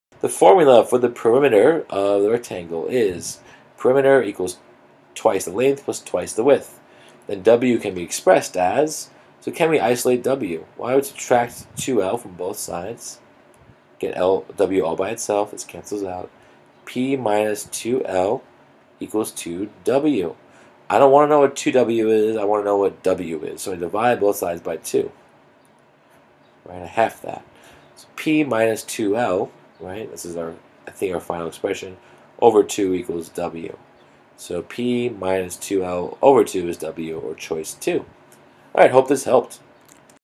Parametric Equations 8 Algebra Regents
In this video we solve for one variable in terms of another